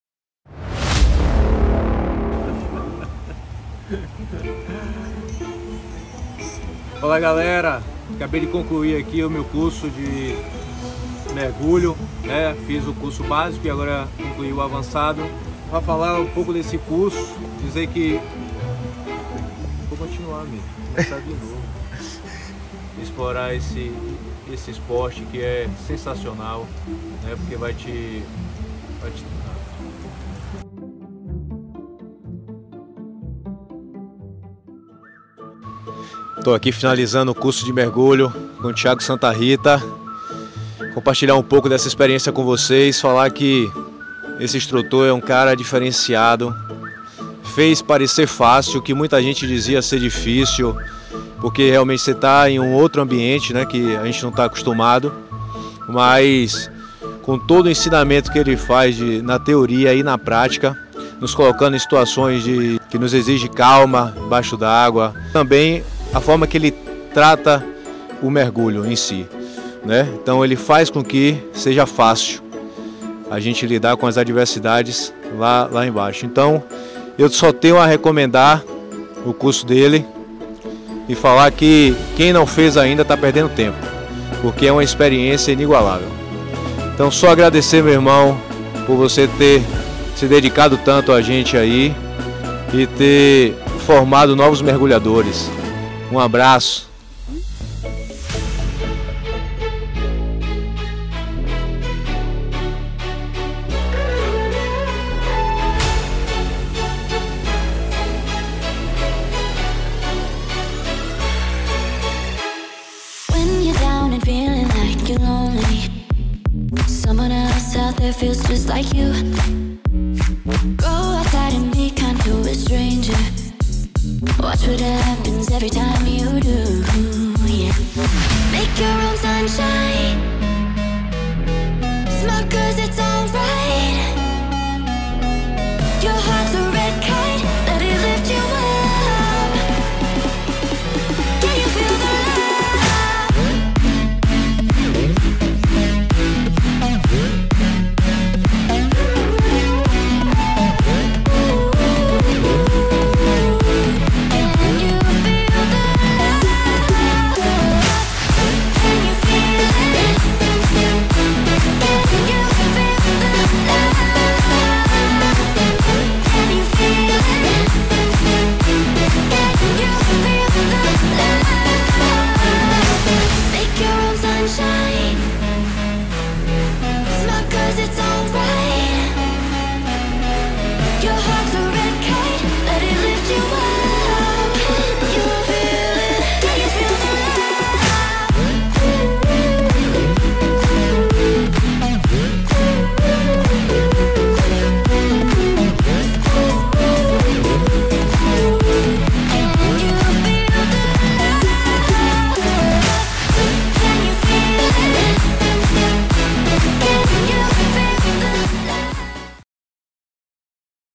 Curso Open Water Scuba Dive de Heider Fiuza - Tiago Santa Ritta
O cara além de advogado, modelo, empresário e bonitão, agora também é mergulhador Scuba.

Entretanto, foi tão bem, que decidiu embarcar na Dive Trip de Abrolhos e fazer o Advanced para ir preparado.

São quase 15 anos de amizade.

Parabéns pela conclusão com sucesso dos seus cursos e mergulhos, irmão.

E obrigado pela confiança, carinho e respeito.

Que venham esses mares para nós desbravarmos.

Osssssss